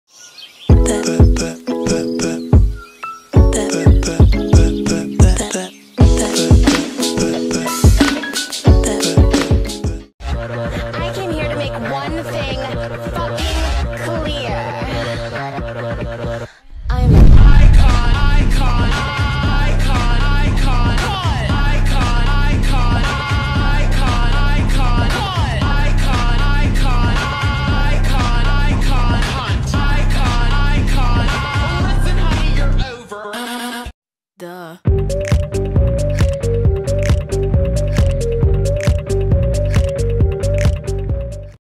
Icon meme | Stupido Potato
Hi everyone! Good morning good evening or afternoon or whatever time it is. I’m back hehe. Basically I recreated Nanak0.o video (I got permission dw) it’s basically the Walmart version of her amazing video, make sure to check out her chanel and follow her on Instagram for wholesome content. That’s all for today, I will post a GLMM soon (hopefully) anyways have a good day or night. Byeee and Peace ✌🏻💗💗💕✌🏻💞💓💞💘💓💗💕💗💗